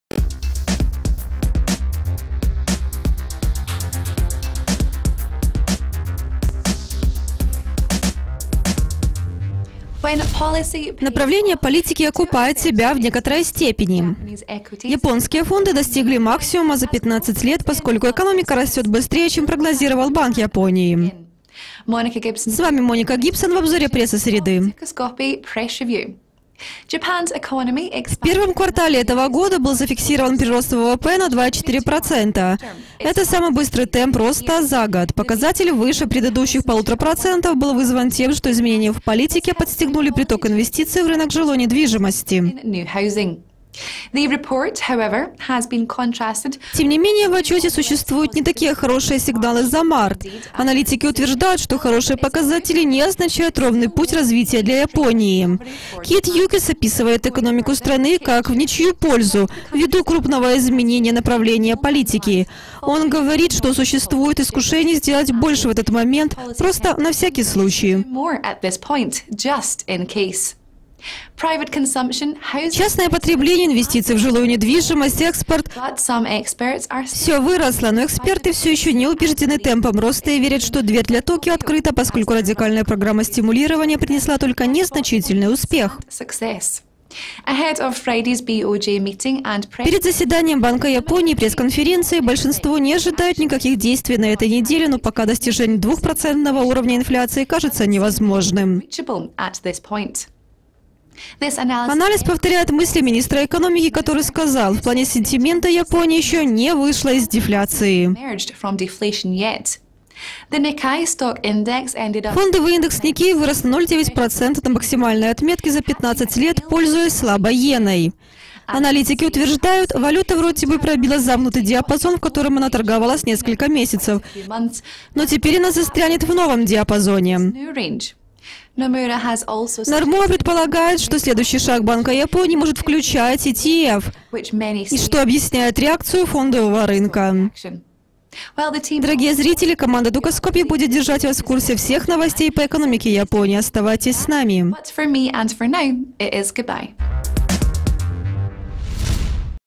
"На всякий случай" - 20.05.2015 - Dukascopy Press Review
Лучший, чем ожидалось, показатель ВВП вовсе не означает, что банк Японии не прибегнет к новым мерам стимулирования.